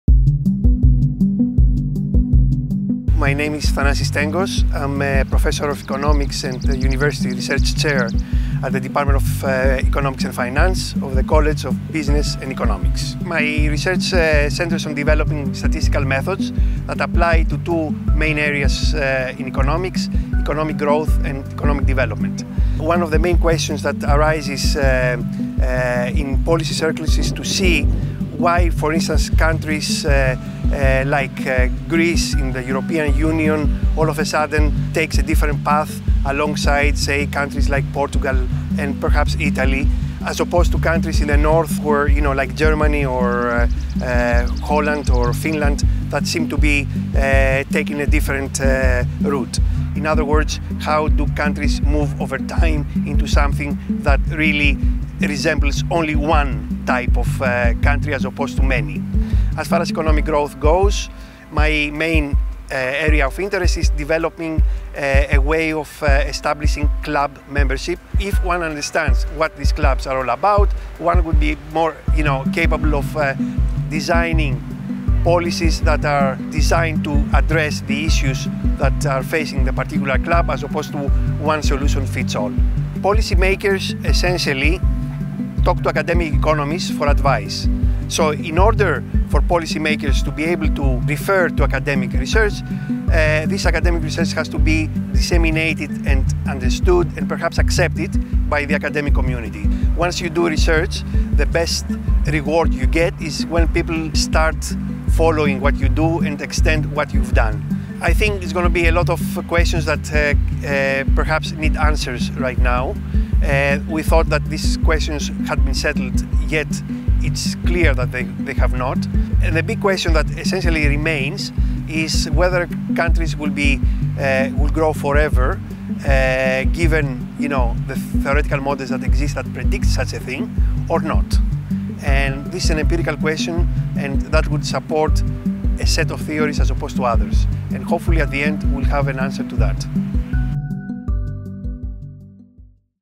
Thanasis Stengos, University Research Chair in Econometrics
Thanasis Stengos discusses his analysis historical economic data to create a better understanding of how and why economies grow.

Video transcript
My name is Thanasis Stengos, I’m a professor of economics and a University Research Chair, at the Department of Economics & Finance, of the College of Business and Economics. My research centers on developing statistical methods that apply to two main areas in economics, economic growth and economic development. One of the main questions that arises in policy circles is to see why for instance countries like Greece in the European Union all of a sudden takes a different path, alongside say countries like Portugal and perhaps Italy, as apposed to countries in the north like Germany, or Holland, or Finland, that seem to be taking
a different route. In other words how do countries move over time into something that really resembles only one type of country as opposed to many. As far as economic growth goes my main area of interest is developing a way of establishing club membership. If one understands what these clubs are all about one would be more capable of designing policies that are designed to address the issues that are facing the particular club as apposed to one sort of fits all. Policy makers essentially talk to academic economists for advice so in order for policy makers to be able to refer to academic research this academic research has to be disseminated and understood and perhaps accepted by the academic community. Once you do research the best reward you get is when people start following what you do and extend what you’ve done. I think there’s going to be a lot of questions that perhaps need answers right now. We thought that these questions have been settled yet it's clear that they have not. The big question that essentially remains is whether countries will be, will grow forever given, you know the economic models that exist and predict such a thing or not and this is an empirical question that would support a set of theories as opposed to others. Hopefully at the end we will have an answer to that.